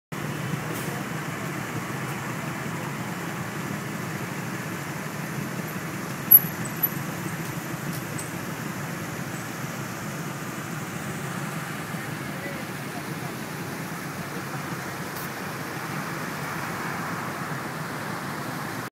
Police road block at Jalan Udini
Police road block at Jalan Udini @ 1-Jun-2012, 1:40am to catch Mat Rempit